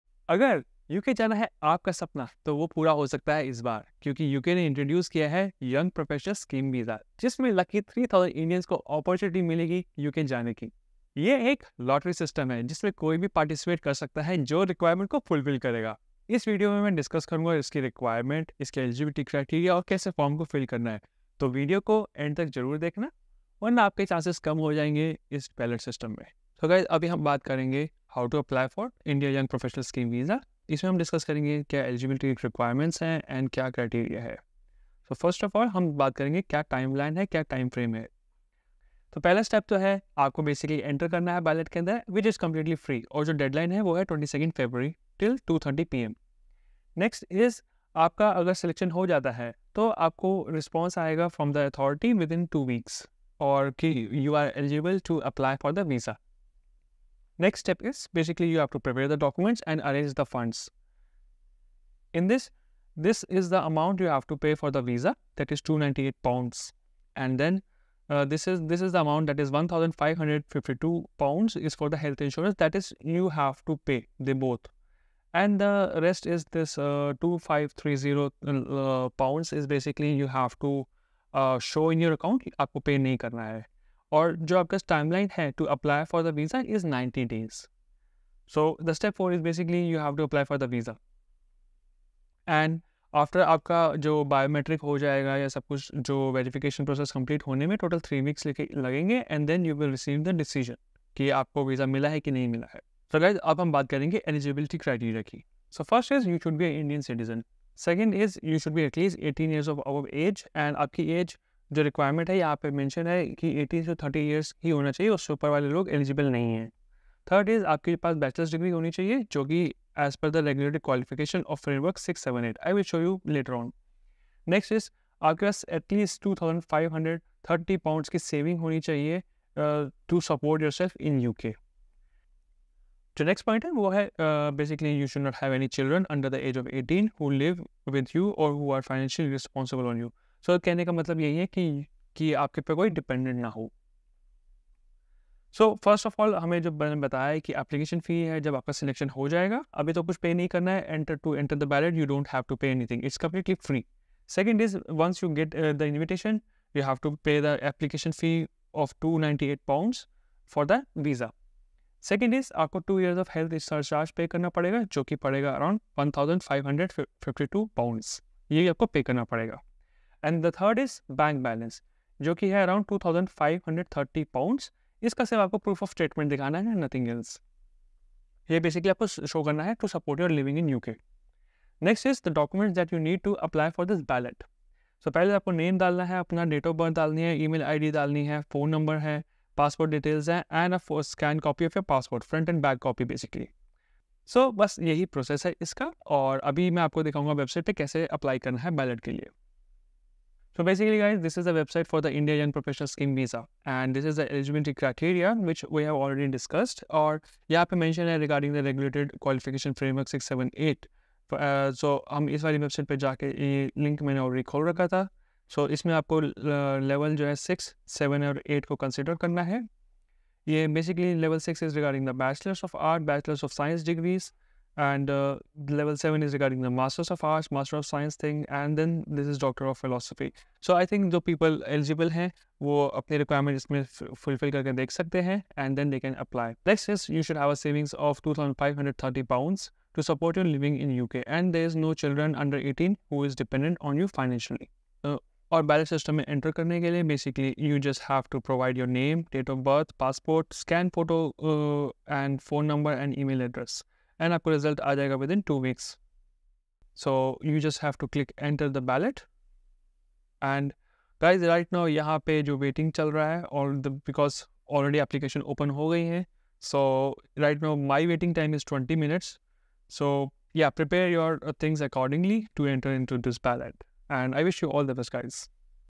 UK Lottery WORK VISA for Indians | Requirements | eligibility criteria
"🌍 Exciting News: The Indian Young Professional Scheme 2024 is here! 🇮🇳🇬🇧 The UK government is offering 3,000 visas for Indians to work, and live abroad.

Join the first ballot opening today, February 20, 2024, at 2:30 PM.

Check the eligibility criteria and application process detailed by the British High Commission in India.

 Don't miss this incredible opportunity!

 📆February 20, 2024, at 2:30 PM.